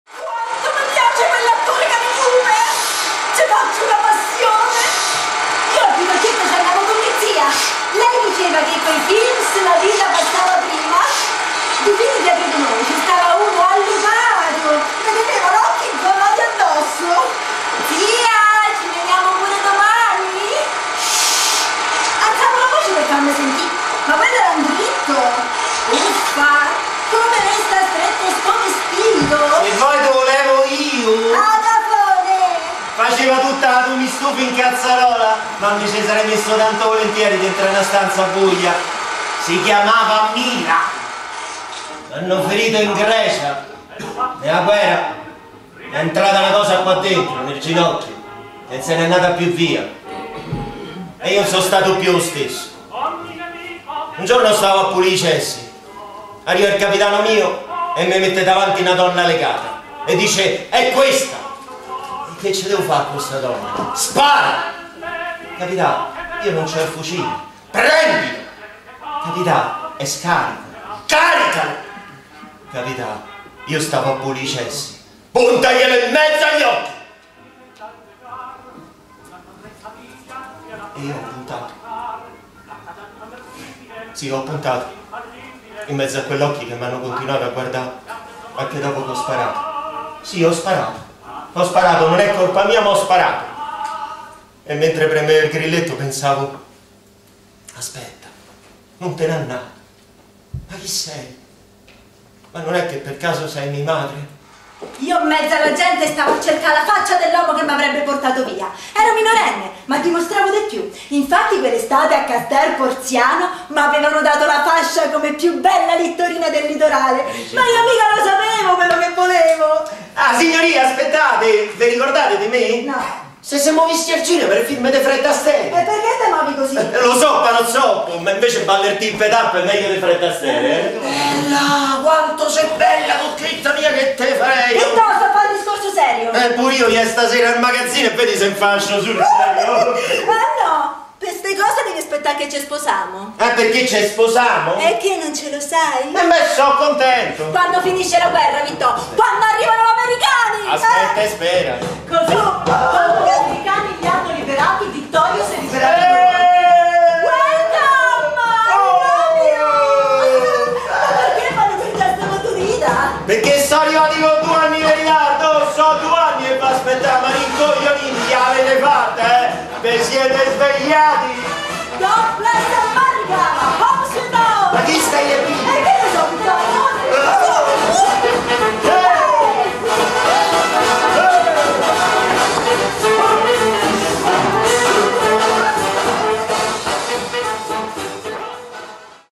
Tribù, Vittorio e Mila
Tribù, di Duccio Camerini. Regia di Luca Milesi. Fabrizio Bordignon e Serena Renzi nei ruoli di Vittorio e Mila